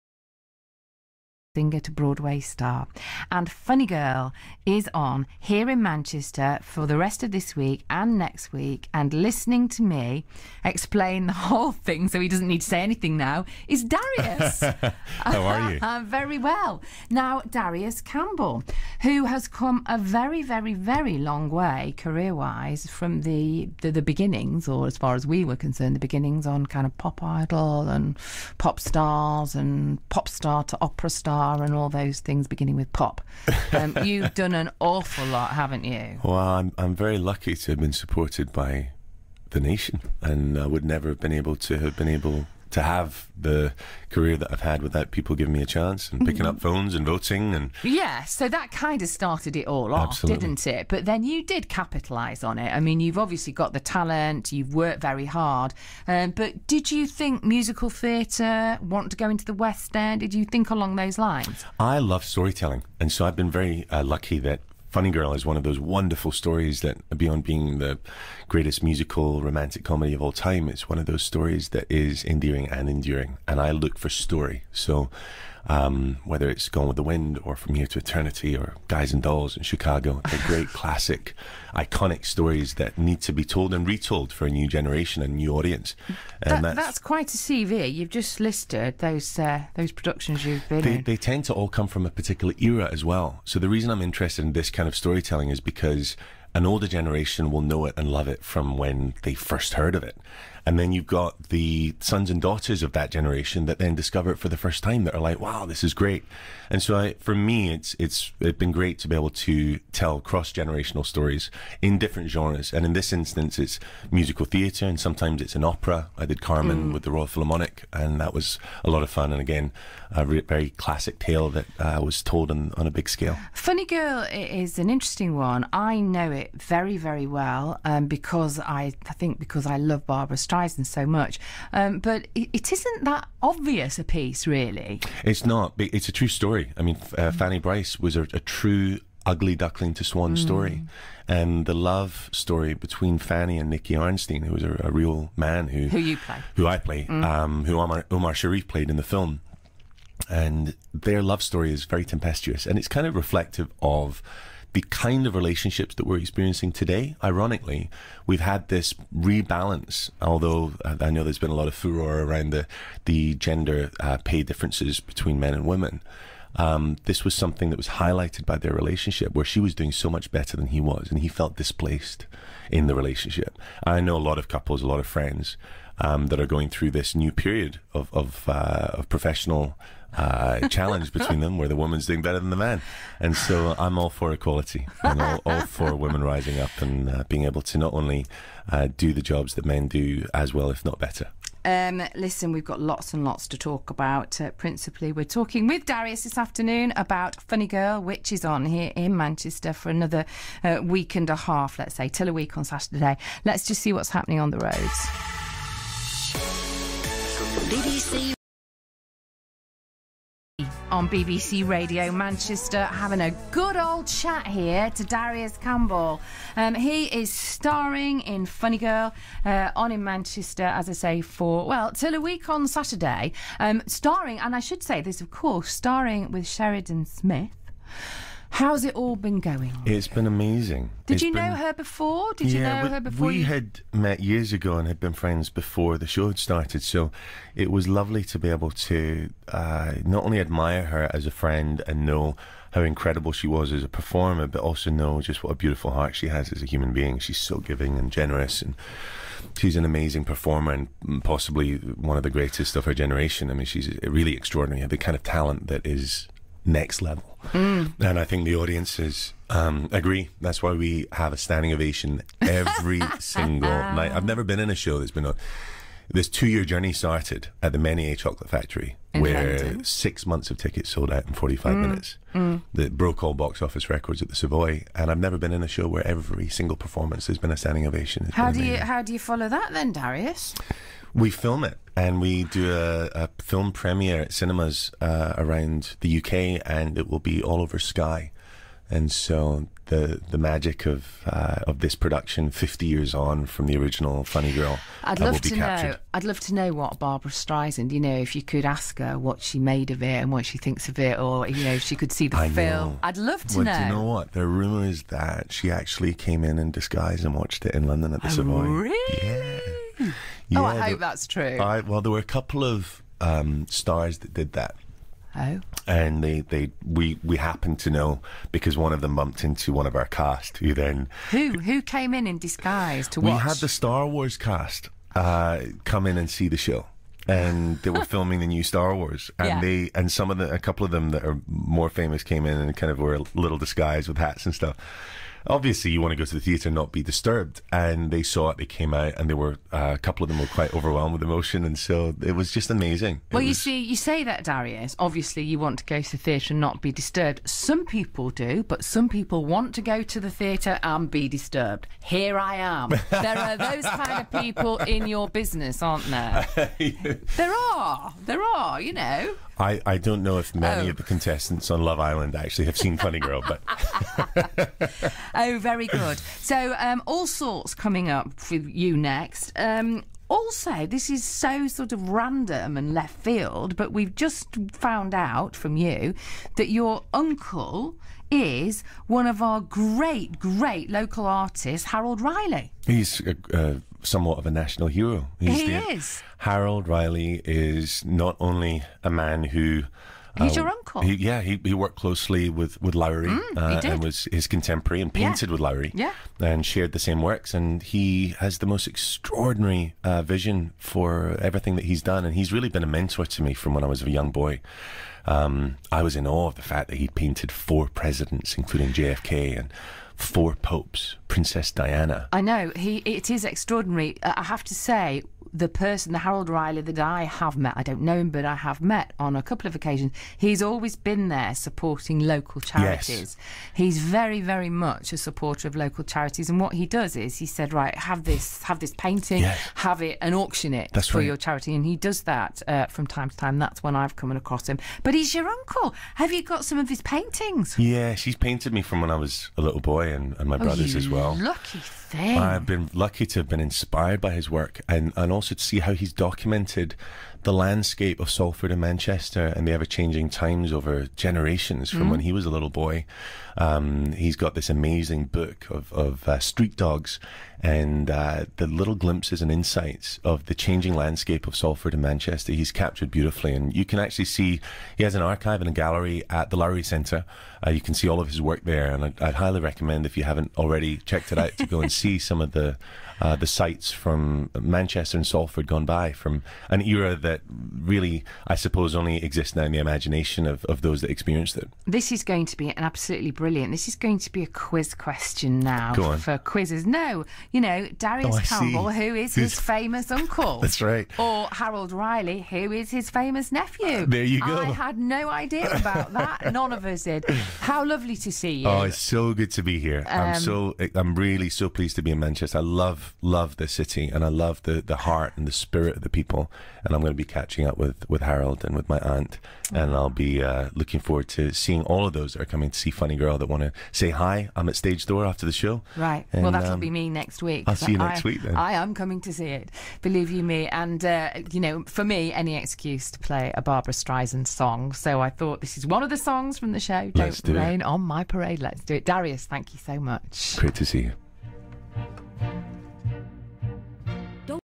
Darius Campbell Danesh - BBC Radio Manchester 'Funny Girl' Interview - August 2017
This interview with Darius was broadcast on BBC Radio Manchester on 10 August 2017.